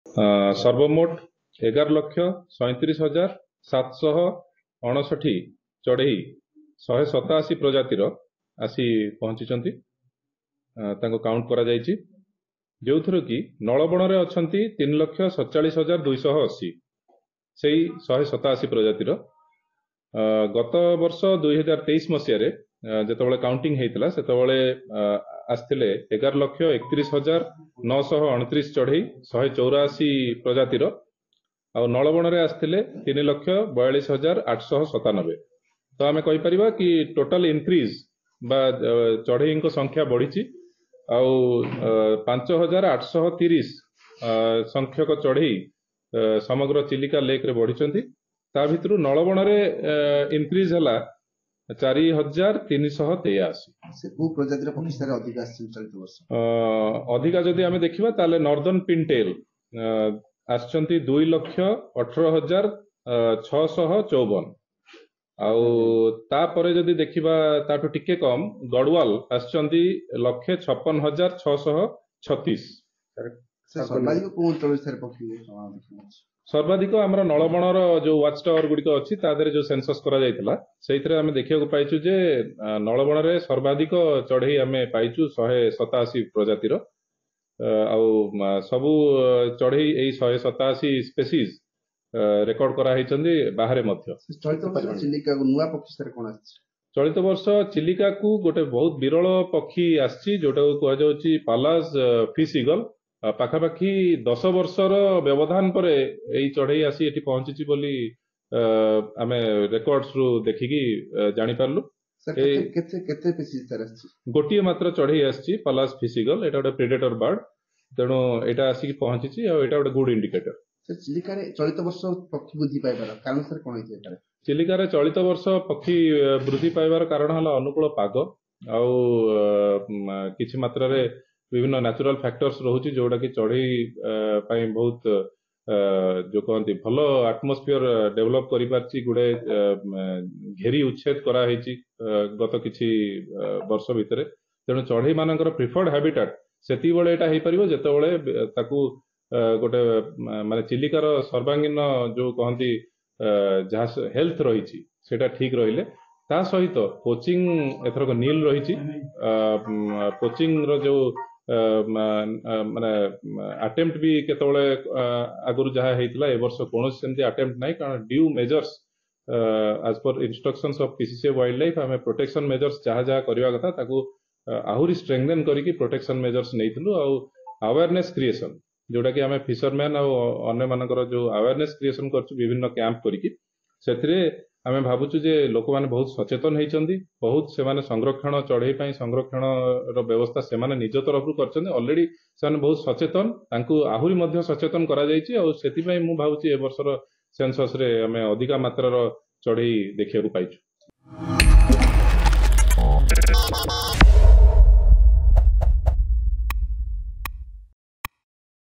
Annual Bird Census Kicks off in Chilika today: ଚିଲିକାରେ ଚଳିତ ବର୍ଷର ବିଦେଶାଗତ ପକ୍ଷୀ ଗଣନା ଶେଷ
ଚିଲିକାରେ ଚଳିତ ବର୍ଷ ର ବିଦେଶାଗତ ପକ୍ଷୀ ଗଣନା ଶେଷ ହୋଇଛି | ଏହି ବର୍ଷ ଚିଲିକାରେ ୧୮୭ ପ୍ରଜାତିର ୧୧ ଲକ୍ଷ ୩୭ ହଜାର ୭୫୯ ଟି ପକ୍ଷୀ ଆସିଛନ୍ତି | ନଳବଣ ରେ ସର୍ବାଧିକ ୩ ଲକ୍ଷ ୪୭ ହଜାର ୨୮୦ ପକ୍ଷୀ ଦେଖିବାକୁ ମିଳିଛି | ସର୍ବାଧିକ ନର୍ଦନ ପିଙ୍କଟେଲ ପ୍ରଜାତି ର ୨ ଲକ୍ଷ ୧୮ ହଜାର ୬୧୪ ଆସିଥିବା ବେଳେ ସର୍ବ ନିମ୍ନ ପେଲିକାନ ପ୍ରଜାତି ଗୋଟିଏ ମାତ୍ର ଦେଖା ଯାଇଛି | ଦଶ ବର୍ଷ ପରେ ପୁଣି ଏକ ନୂଆ ପ୍ରଜାତି ର ପକ୍ଷୀ ପଲାଜ ଫିସ ଈଗଲ ଚଳିତ ବର୍ଷ ଦେଖିବାକୁ ମିଳିଛି | ଗତ ବର୍ଷ ଚିଲିକାରେ ୧୮୪ ପ୍ରଜାତି ର ୧୧ ଲକ୍ଷ ୩୧ ହଜାର ୯୨୯ ଆସିଥିବା ବେଳେ ଚଳିତ ବର୍ଷ ୫ ହଜାର ୮୩୦ ଟି ପକ୍ଷୀ ବୃଦ୍ଧି ପାଇଛି |
About Channel:-